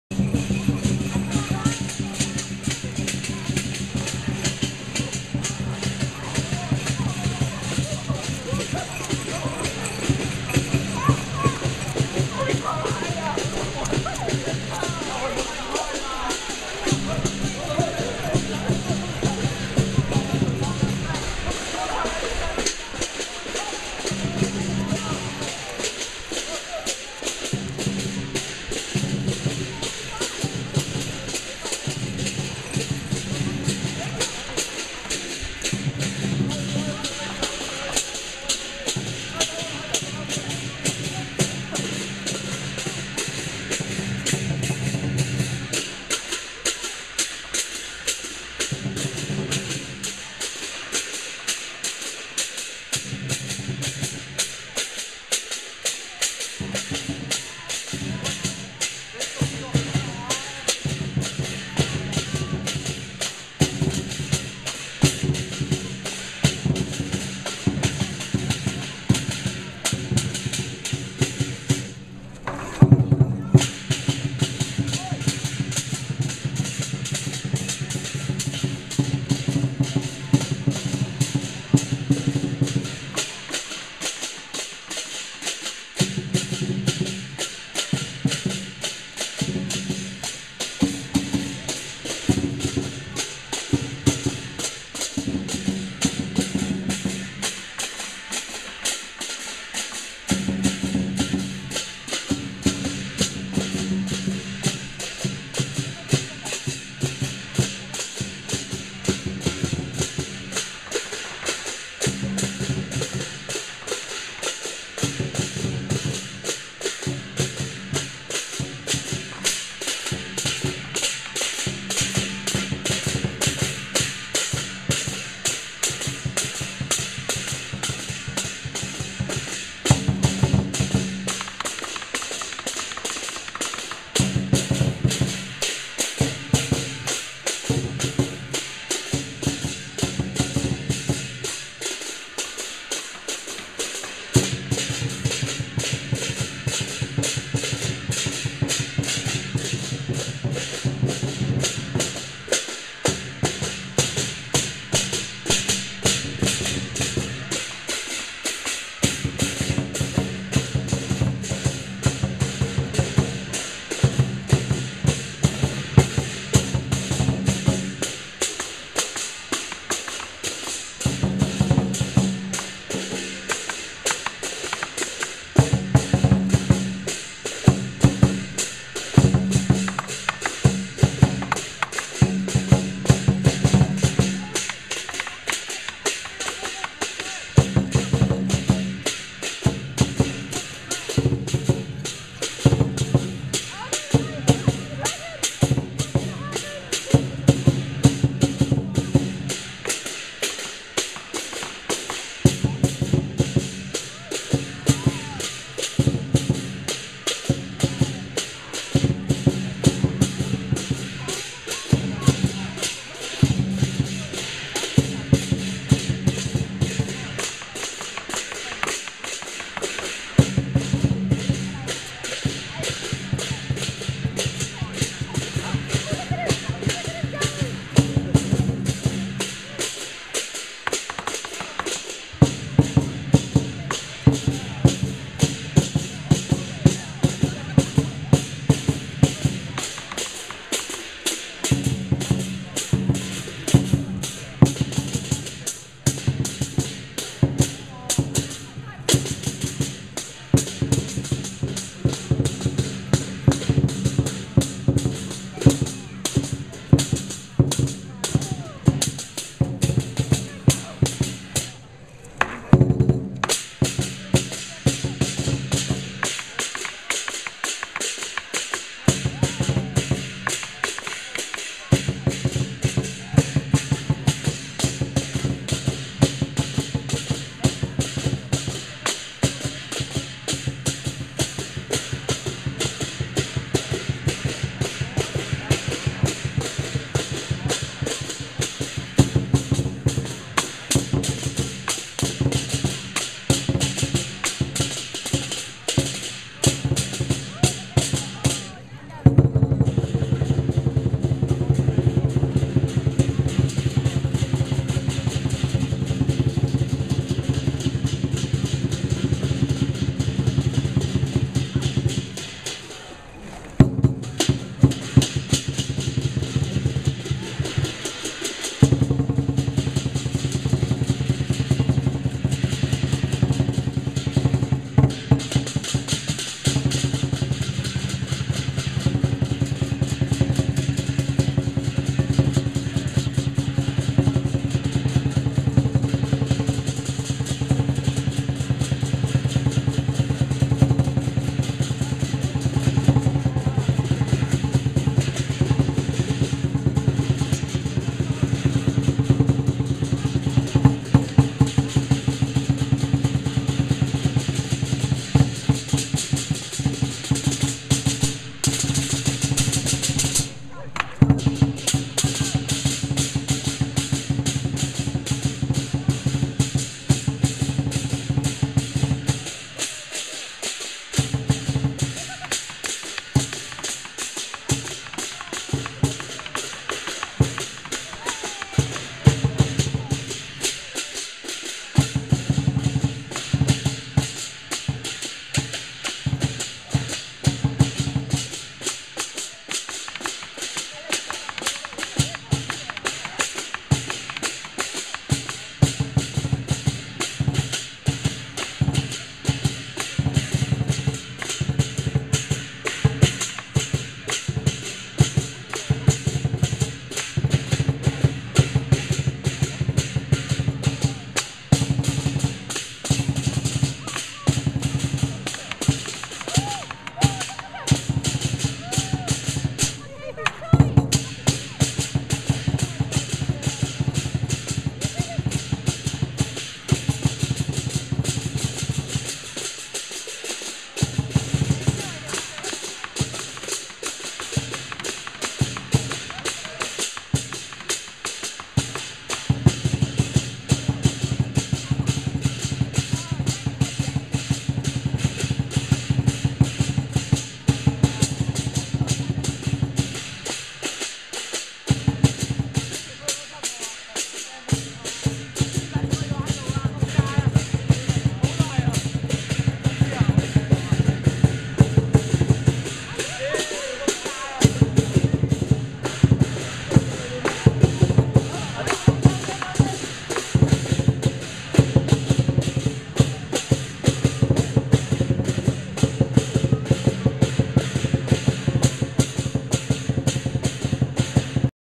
Chinese New Year of Dragon Bedford Square Exeter UK
Feb 2012 I think nthe dragon usually lives in Poole. continues on university campus. more later, this is not edited.